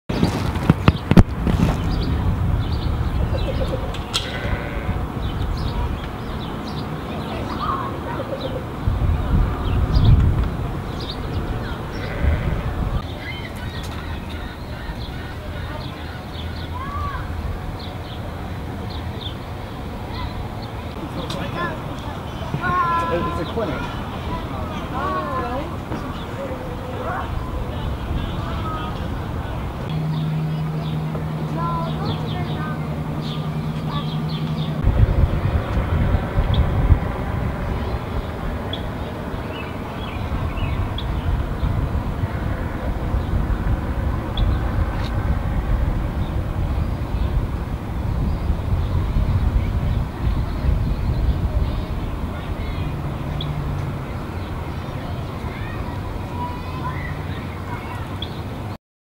Least Bittern song and catching fish
Least Bittern calling and catching fish . Video taken at Lincoln Park Zoo's South Pond.